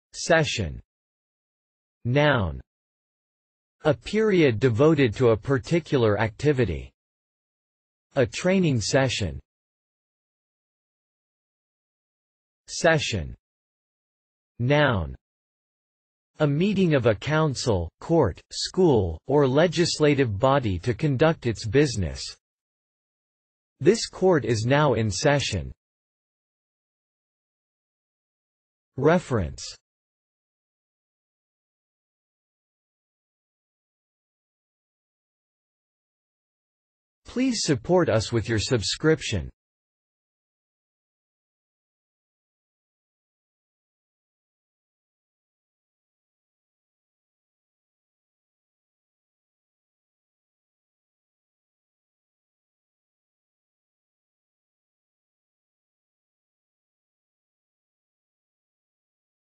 Session | Meaning of session 📖 📖 📖 📖 📖
See here, the meanings of the word session, as video and text.

session (noun)
    A period devoted to a particular activity.
        A training session.

session (noun)
    A meeting of a council, court, school, or legislative body to conduct its business.
        This court is now in session.

Reference:
    session